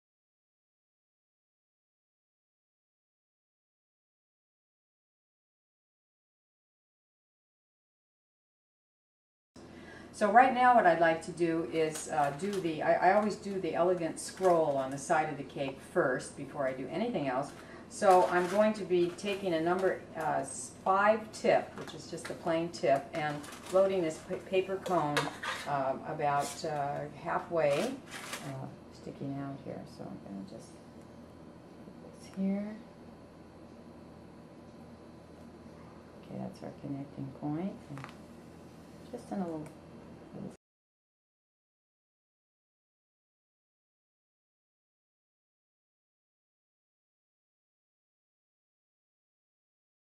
Whipped Cream - Piping Side Scrolls and Bottom Border - Preview
Baking & Cake Art Academy's Preview for Whipped Cream - Piping Side Scrolls and Bottom Borde with Susan Holtz.